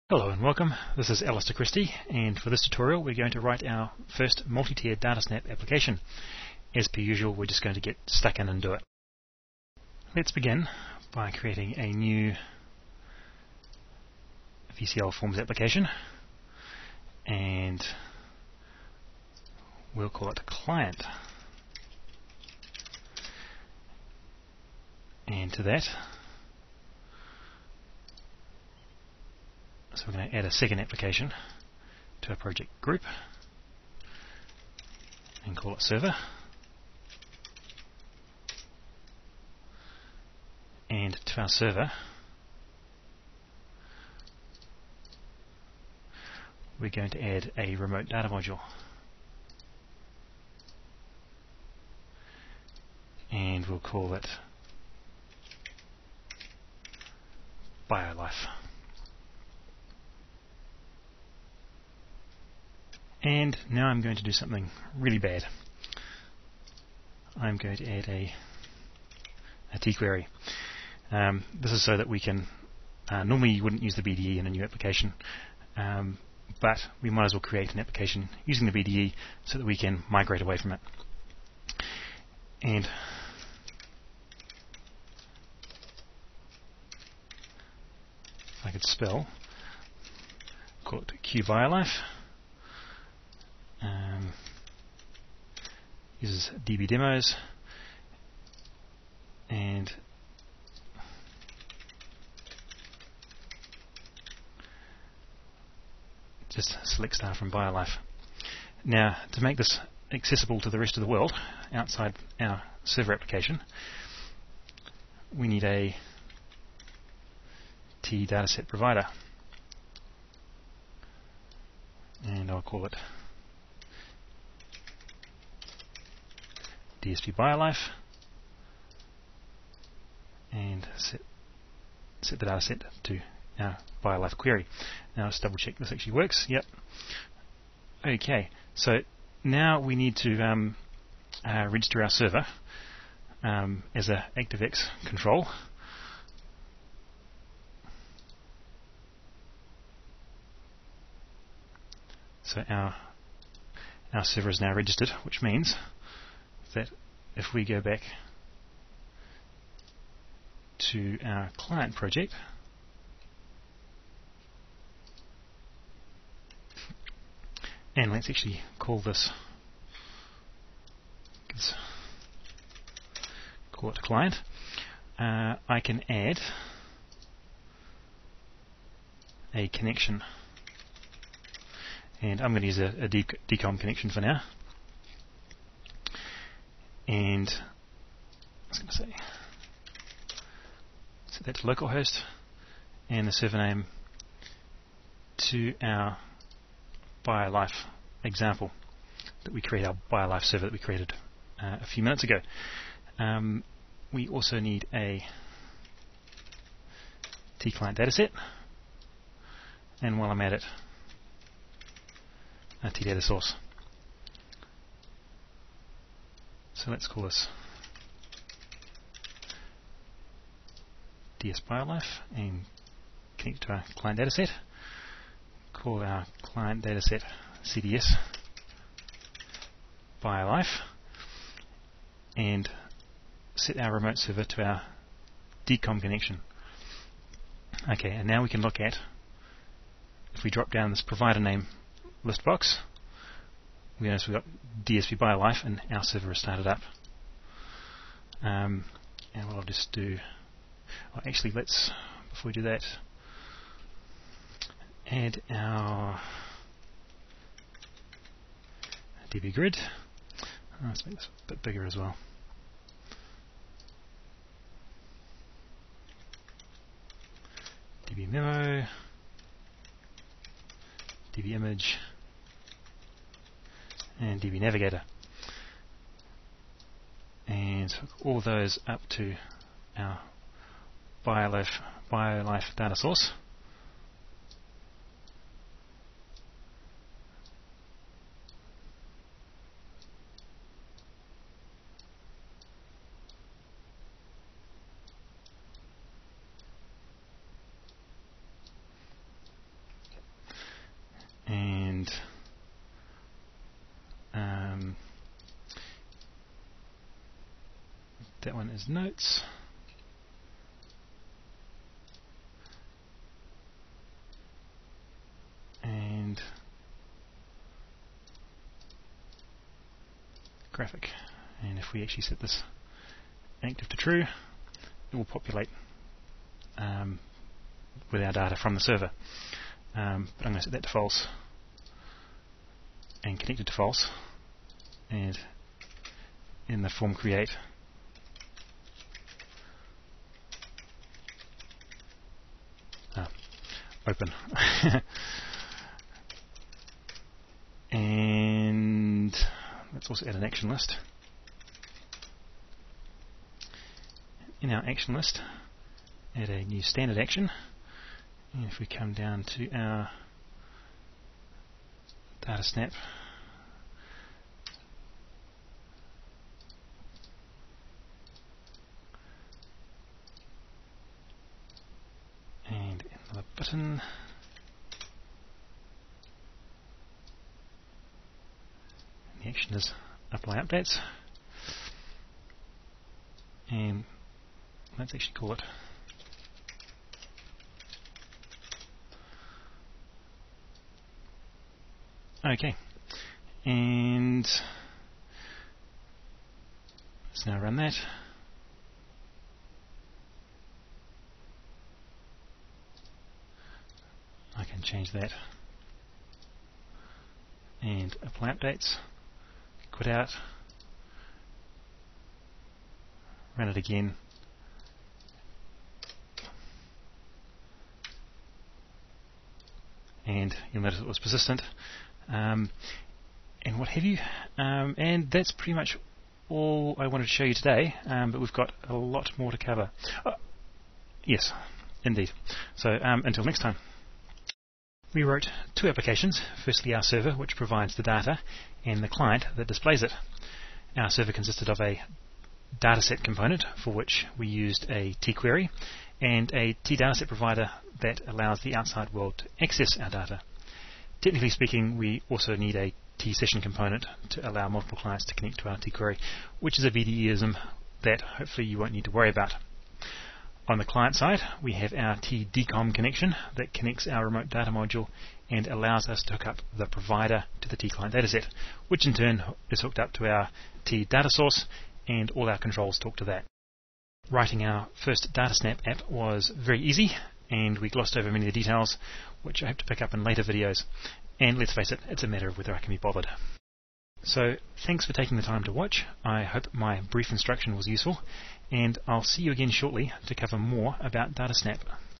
Delphi Training Movie #35 - DataSnap #1
This is our first movie to look at the DataSnap technology in Delphi which allows us to fairly easily write multi-tier applications.